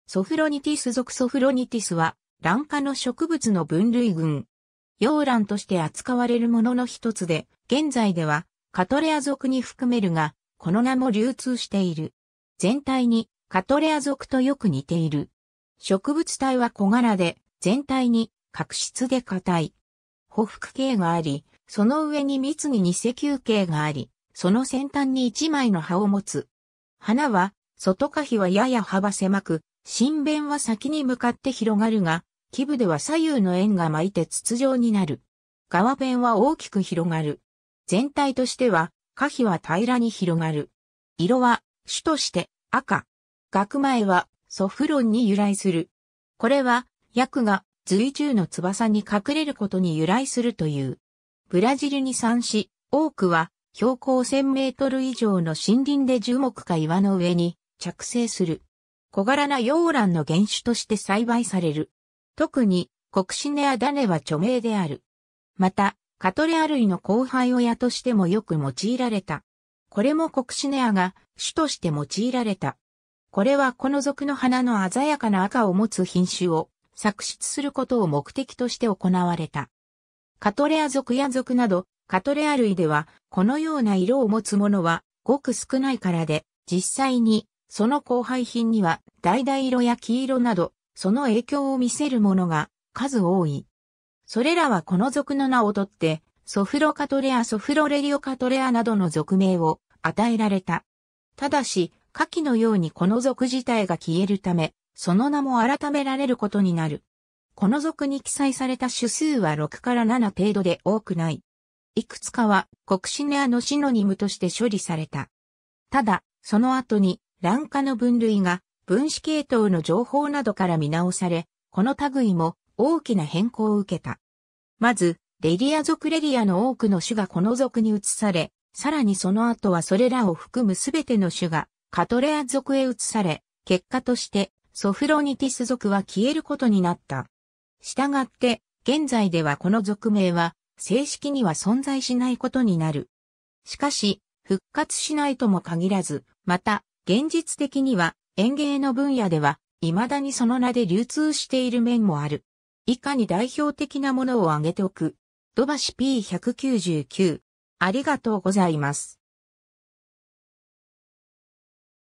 ソフロニティス属
ソフロニティス属　Sophronitis は、ラン科の植物の分類群。
洋ランとして扱われるものの一つで、現在ではカトレア属に含めるが、この名も流通している。
 全体にカトレア属とよく似ている。
植物体は小柄で、全体に革質で硬い。
匍匐茎があり、その上に密に偽球茎があり、その先端に一枚の葉を持つ。
 花は外花被はやや幅狭く、唇弁は先に向かって広がるが、基部では左右の縁が巻いて筒状になる。
側弁は大きく広がる。
全体としては花被は平らに広がる。
色は主として赤。
 学前は sophron （慎み深い・上品な）に由来する。
これは葯がずい柱の翼に隠れることに由来するという。
 ブラジルに産し、多くは標高1000m以上の森林で樹木か岩の上に着生する。
 小柄な洋ランの原種として栽培される。
特にコクシネア（またはコッキネア）種は著名である。
 また、カトレア類の交配親としてもよく用いられた。
これもコクシネアが主として用いられた。
これはこの属の花の鮮やかな赤を持つ品種を作出することを目的として行われた。
カトレア属や属など、カトレア類ではこのような色を持つものはごく少ないからで、実際にその交配品には橙色や黄色など、その影響を見せるものが数多い。
それらはこの属の名を取ってソフロカトレア・ソフロレリオカトレアなどの属名をあたえられた。
ただし、下記のようにこの属自体が消えるため、その名も改められることになる。
 この属に記載された種数は6-7程度で多くない。
いくつかはコクシネアのシノニムとして処理された。
 ただ、その後にラン科の分類が分子系統の情報などから見直され、この類も大きな変更を受けた。
まず、レリア属 Laelia の多くの種がこの属に移され、さらにその後はそれらを含むすべての種がカトレア属へ移され、結果としてソフロニティス属は消えることになった。
 従って、現在ではこの属名は正式には存在しないことになる。
しかし、復活しないとも限らず、また、現実的には園芸の分野では、未だにその名で流通している面もある。
以下に代表的なものをあげておく。
 ^ 土橋(1993)p.199